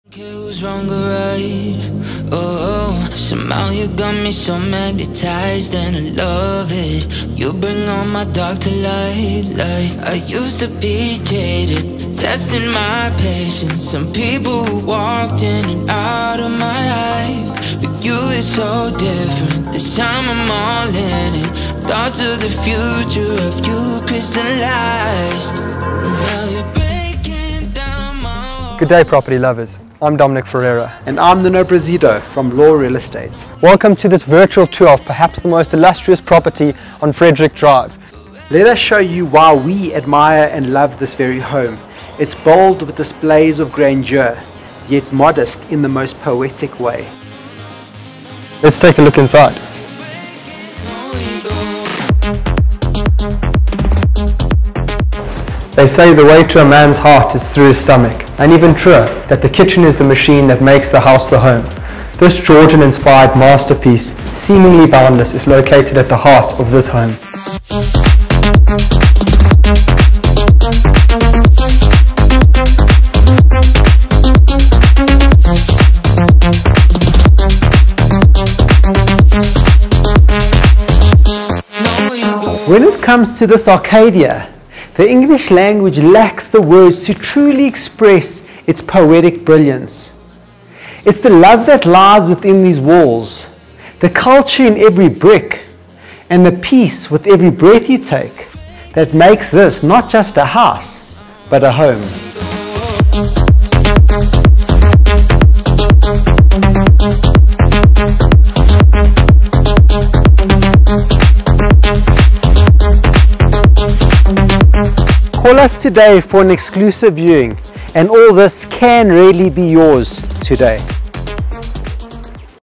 MAGNIFICENT MANSION in Northcliff 😍
A LETTER FROM JANE AUSTEN:
Dear reader…

You must allow me to tell you how ardently I admire and love this very house – it’s bold with displays of grandeur yet modest in the most poetic way.

The most dignified entrance welcomes you with its statuesque pillars... I can hardly write as I enter, I am every instant seeing something that completely and wholly overpowers me – the way every single piece of furniture, every single wall, every single tile, every item of décor, speaks the language of the relentless aesthetic this house mothers.

To my left, is a courtly sitting room with an office tucked away in its corner – this is where I picture my older sister playing the piano as I lose myself in a well written book with my mother in the next room preparing the table for dinner. An adequate dining area I might add – its not only sizeable enough to fit our large family, it also splays out on to an alluring, outdoor passage that takes you to a myriad of places.

One of these places being the ever-favoured, Georgian kitchen. I have yet to find such a boundless kitchen with both its dynamism and efficacy – after all it has integrated appliances along with a gas stove. The kitchen door allows you to walk out onto the passage to a court area perfect for catching the warm sun on a cool morning, while verdancy towers over you in the most impassioned way.

On the right side of the court are doors that allow for a sizably large entertainment room, with a pizza oven, a traditional braai, and a spit braai. And behind the court lies another entertainment room with a massive, sublime bar.

Upstairs, the main bedroom reminds you what it feels like to be crossed in total serenity…from it’s intimately crafted fireplace, to the perfectly labyrinthine en-suite bathroom that provides access to the balcony with bewitching views. The other two capacious bedrooms upstairs have their own balconies that overlook the front garden that is nothing short of empyrean – making each room a sanctuary.

At last, the part of the house that pierces my very soul…the garden that charities this house with its compelling presence. To hear the susurration of the gallant water feature on the pool as I stroll down the stairs of the patio, running my hand along the meticulous railing, my aim being to sit in the shade on a fine day, and look upon verdure… it would be the most perfect refreshment. I truly pity those who have not been given a taste for nature early in life, for she is a goddess who can create harmony and winsomeness – and some of her best work lies here in this very garden.

As if this house is not already a complete haven, it also contains a cinema room downstairs that is linked to a wine cellar, maids’ quarters, yet another office, a gym area, a guest bedroom, as well as a double-sided fireplace where the bar room shares a wall with one of the ever-so-debonair sitting rooms.

One could write a novel about this arcadia; the English language lacks the words to truly express it’s poetic brilliance. Just remember my dear reader, love lies in these walls, culture in every brick, and peace in every breath you take…this is a home, not just a house.